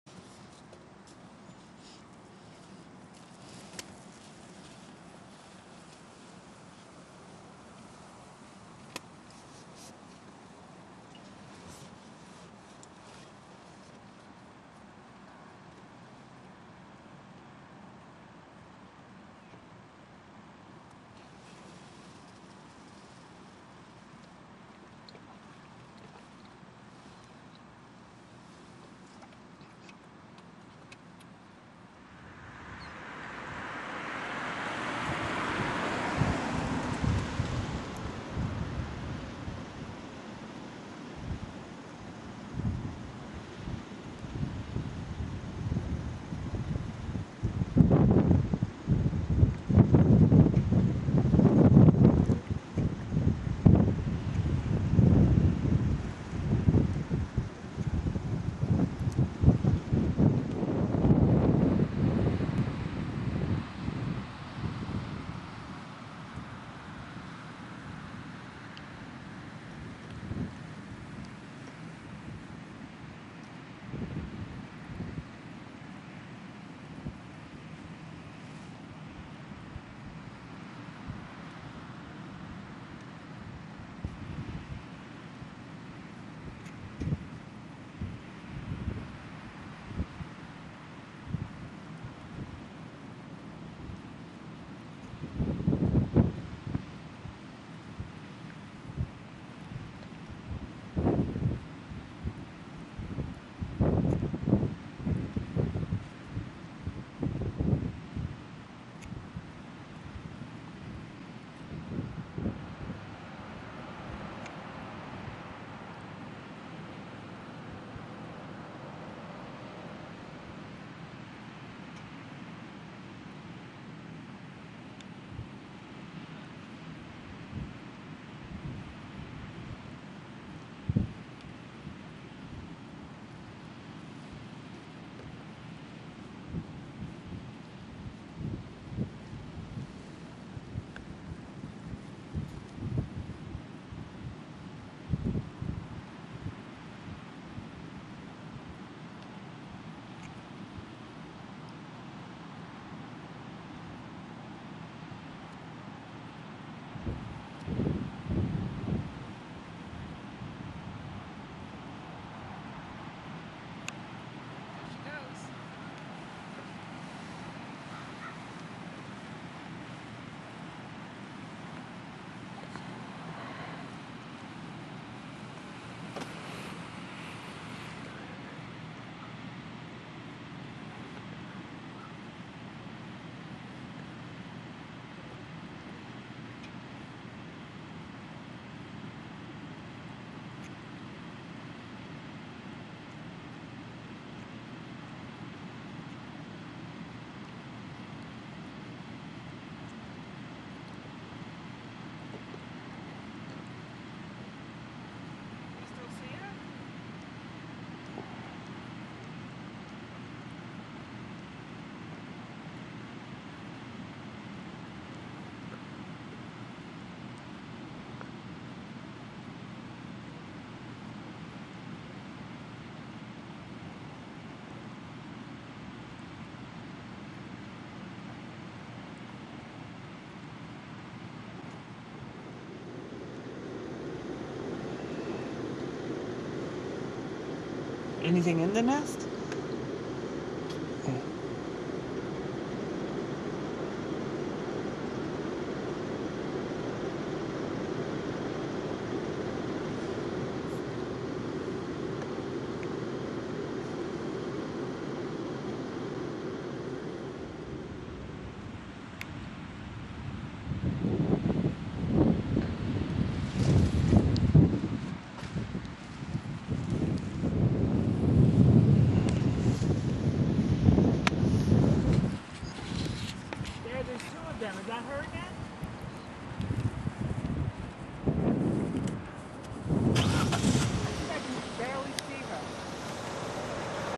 Bald Eagle (Haliaeetus leucocephalus) Amazing fly in the end
Wild bald eagle in Grahamsville, NY.  Its really amazing to get to watch the eagle in its natural envirenment It was really cold and windy so the video came out a bit shaky.